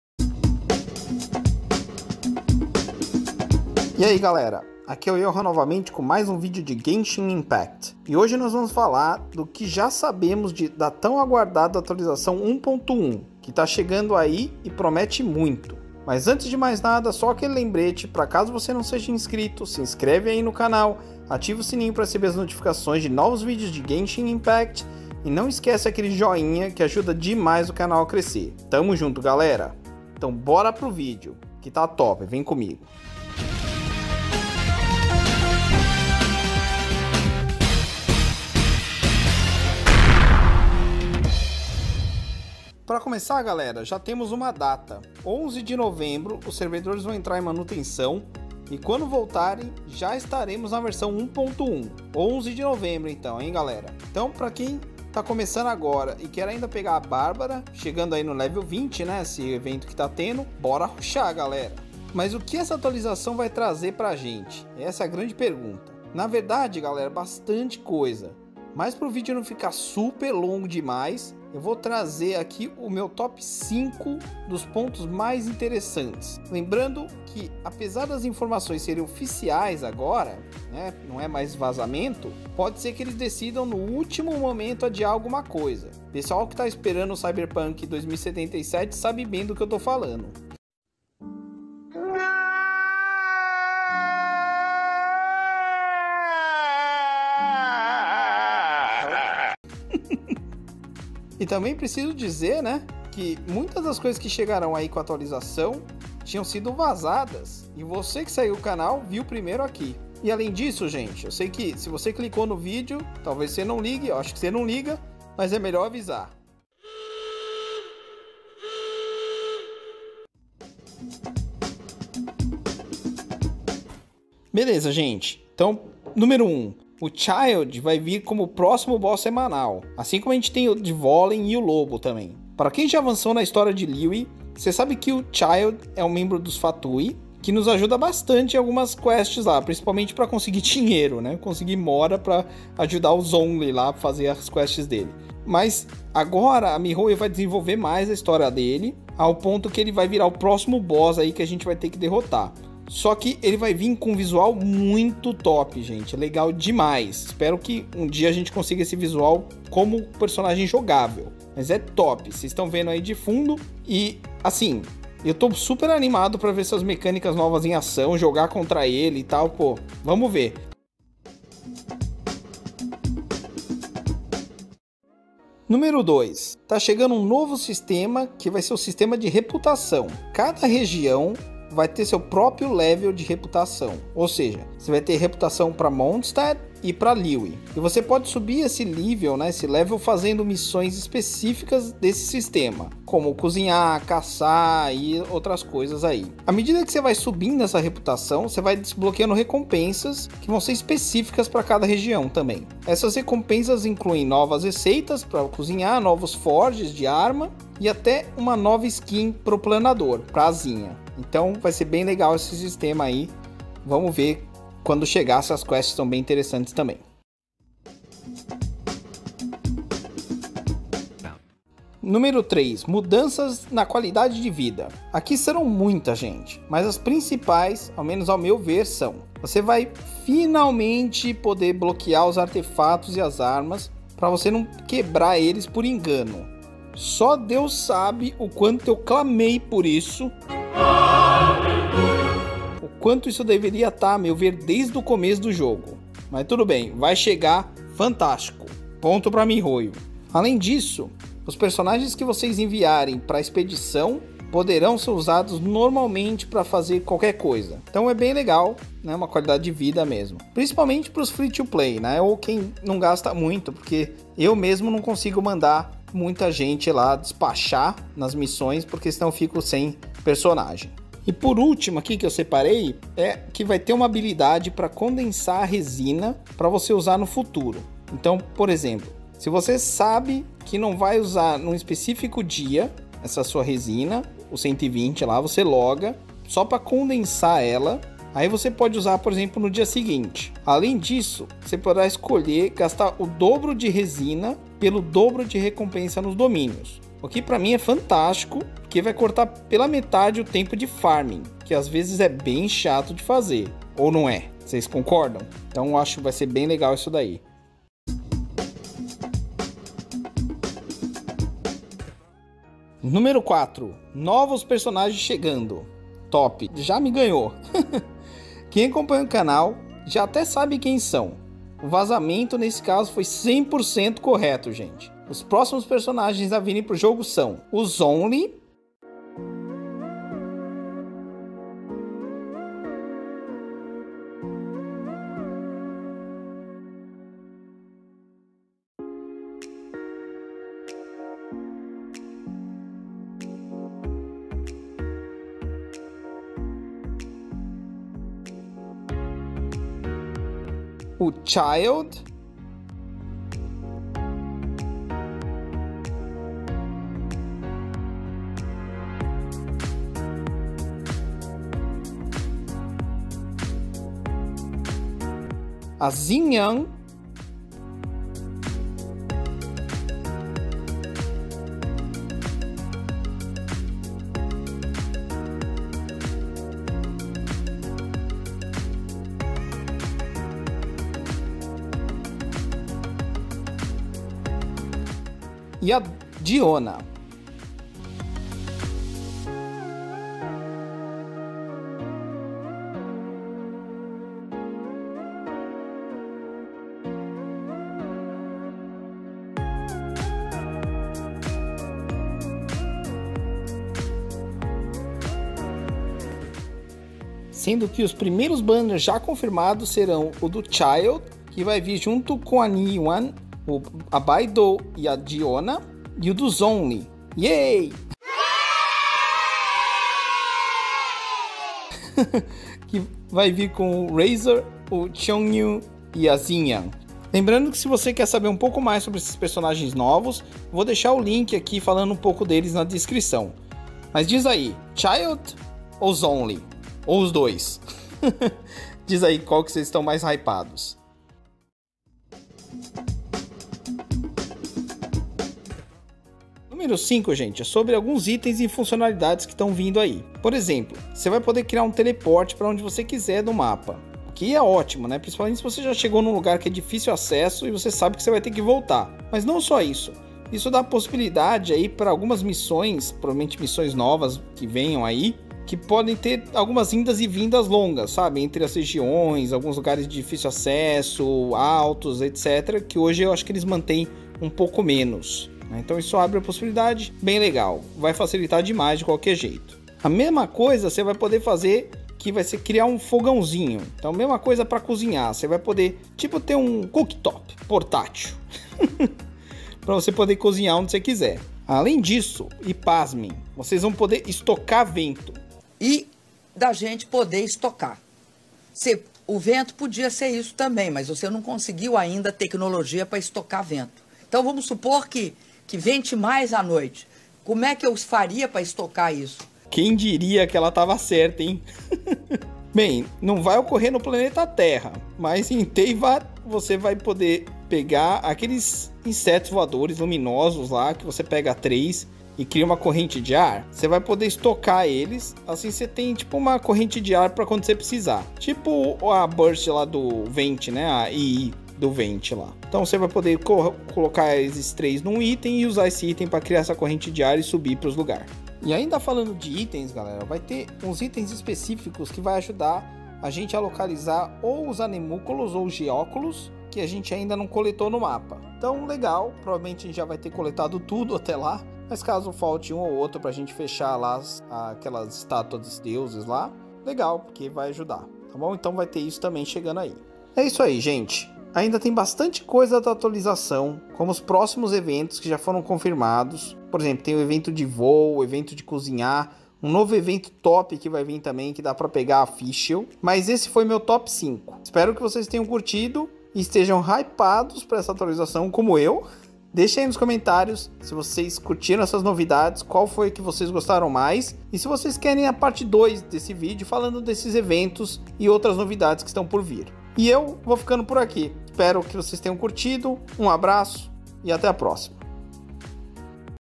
Genshin Impact - vai ficar ainda melhor!
Chegando a atualização do jogo logo logo e parece que está top! Trazendo novos personagens e muito mais! Dá uma conferida no vídeo para ficar por dentro e deixe nos comentários se vocês estão tão ansiosos quanto nós!

Isso ajuda o canal a crescer e continuar a trazer conteúdo de mobile games.
Acesse também nosso Instagram: @yuyogamersoficial

Muito obrigado por nos assistir!